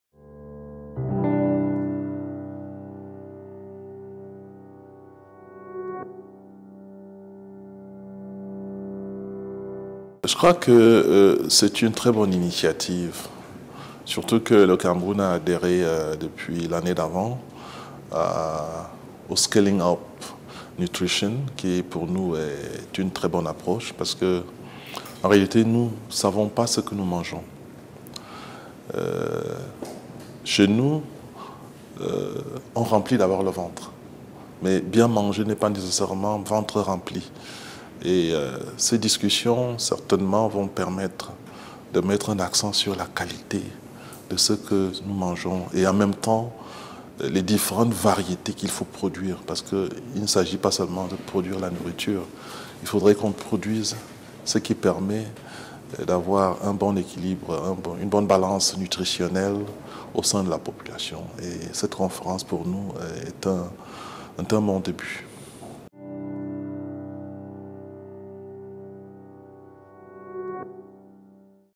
Cameroon's Minister of Agriculture & Rural Development supports ICN2
M. Menye Essimi, Cameroon's Minister of Agriculture & Rural Development supports the Second International Conference on Nutrition which will take place at FAO Headquarters in Rome, 19-21 November 2014.